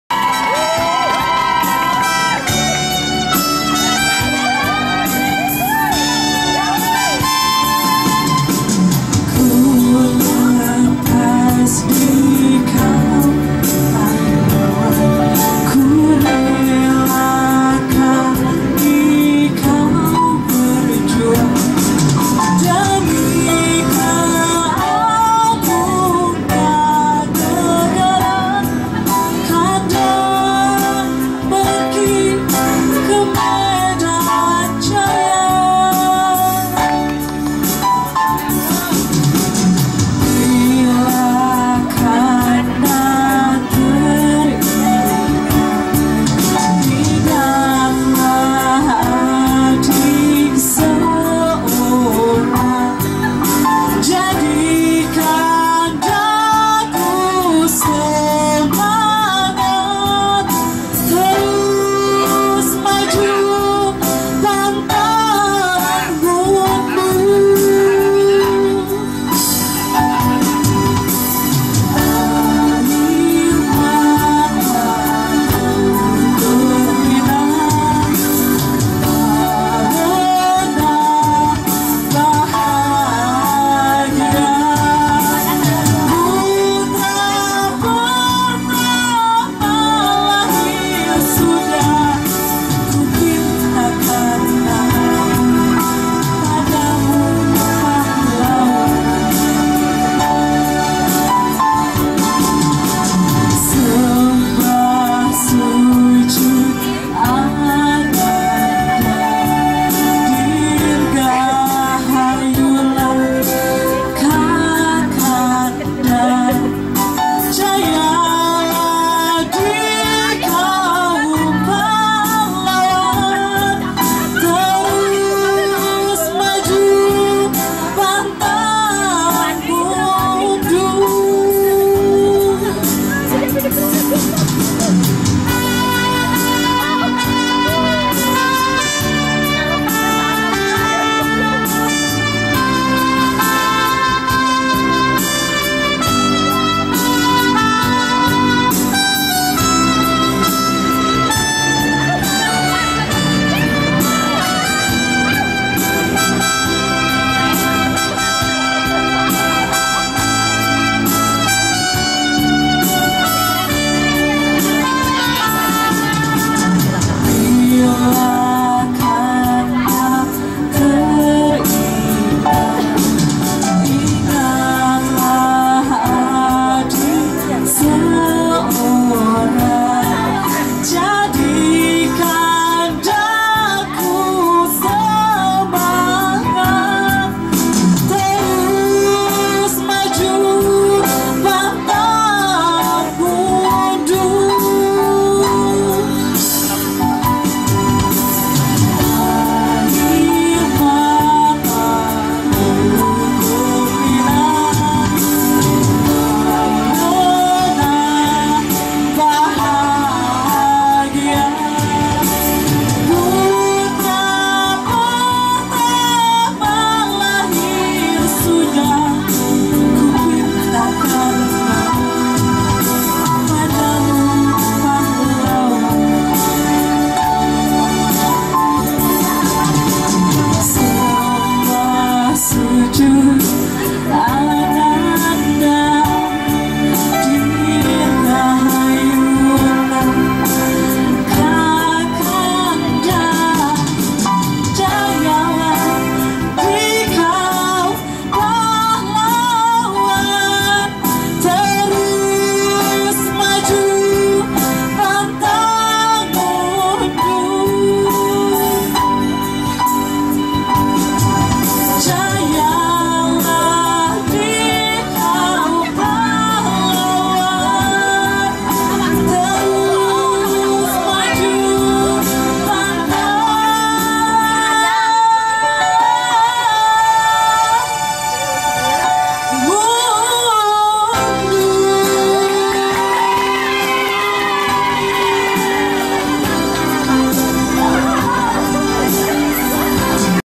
Gebyar Lagu Perjuangan 4); HUT RI 71 Kecamatan Haurwangi
Menyambut HUT RI ke 71; PHBN Tingkat Kecamatan Haurwangi menyelenggarakan Lomba Lagu Perjuangan